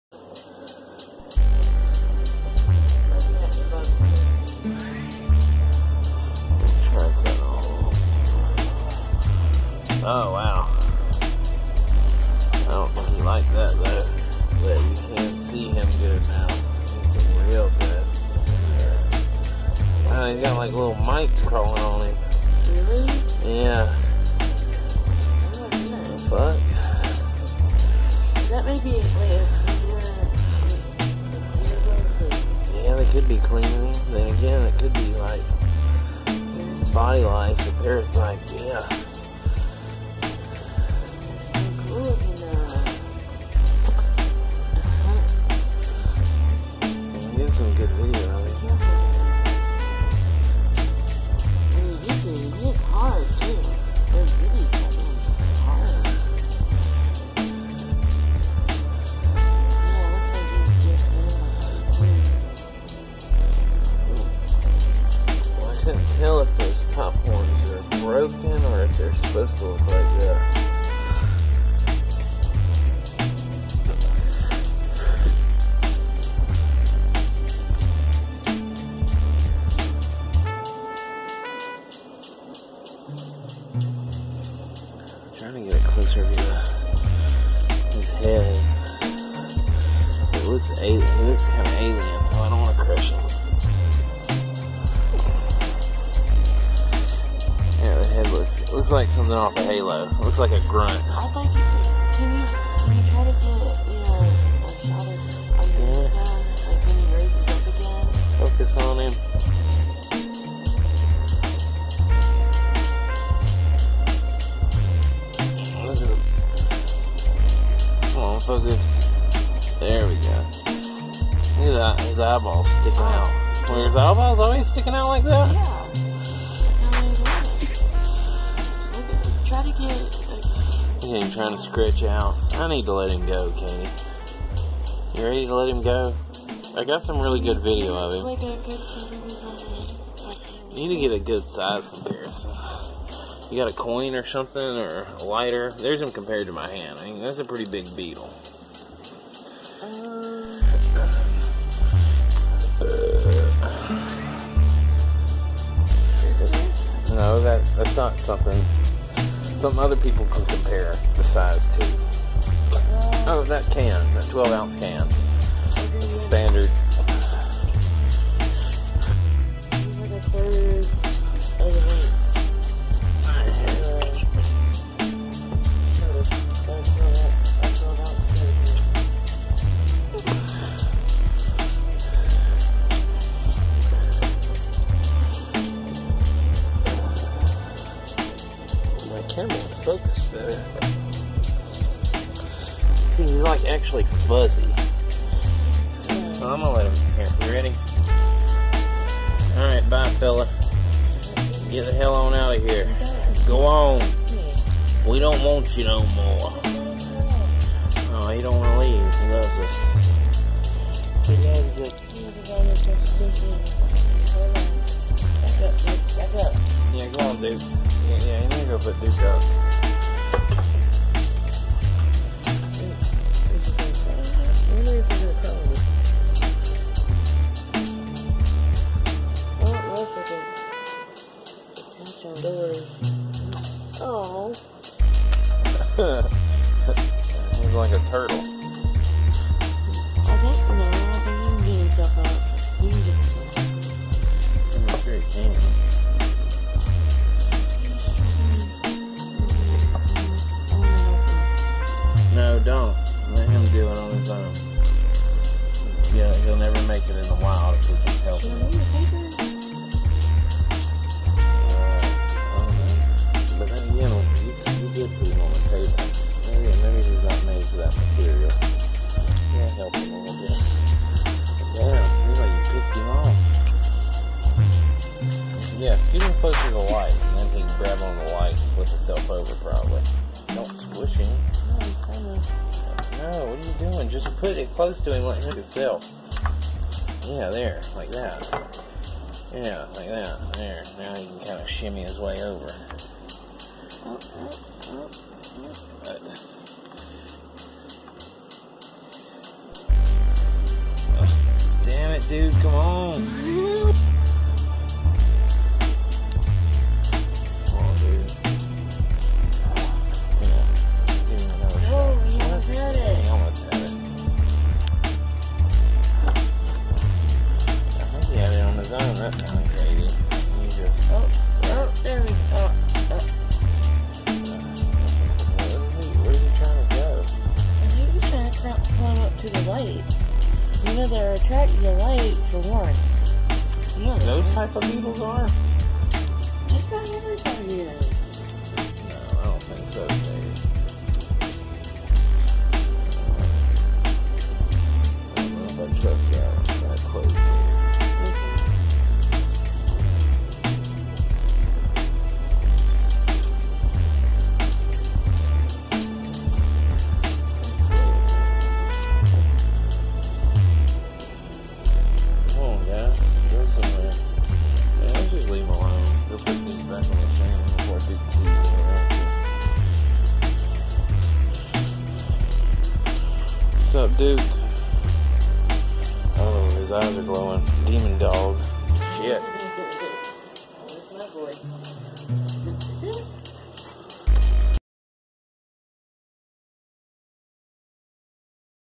LARGEST AND MOST COMMON BEETLE IN THE US! THE OX BEETLE!
The ox beetle also known as the elephant beetle is the largest and most common beetle in the USA. There are two kinds of males with different horns, this beetle in the video is a minor male ox beetle, the major male ox beetle has points on it's top horns and the top two horns of the minor ox beetle are stubbed or flattened. The female ox beetle doesn't have horns.